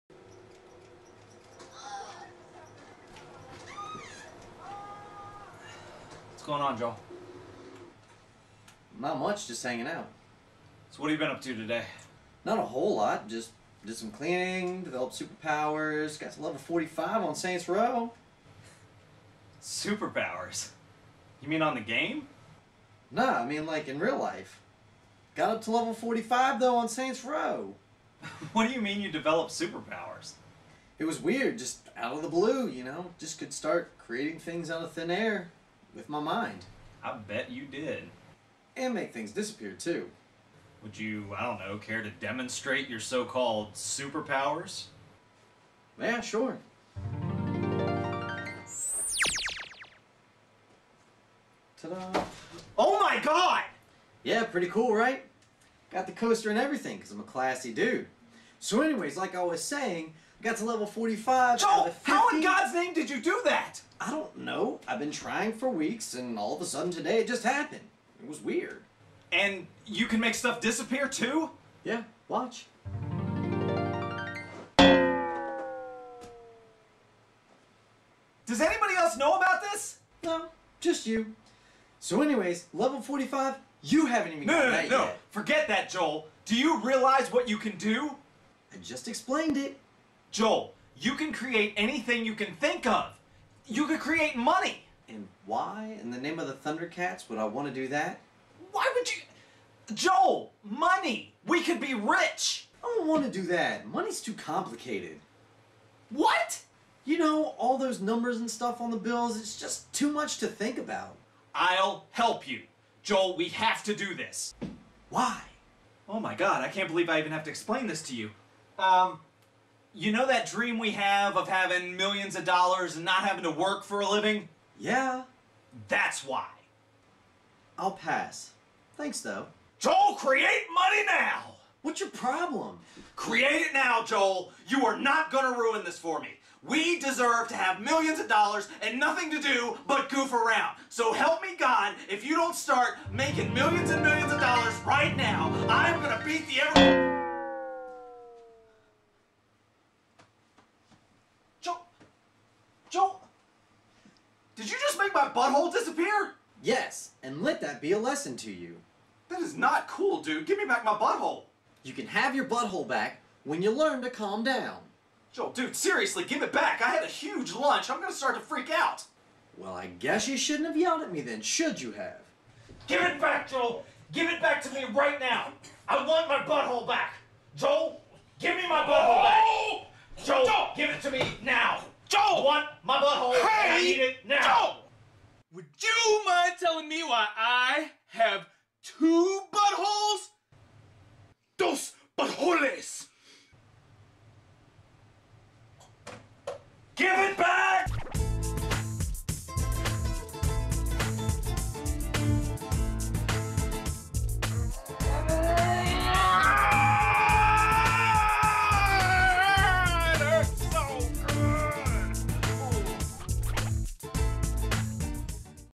Super Powers
Joel shows Andy his newly developed super powers, but will he use them for good... or for evil... or for nothing.

TripleThreatProdux 2012

WE DO IT ALL!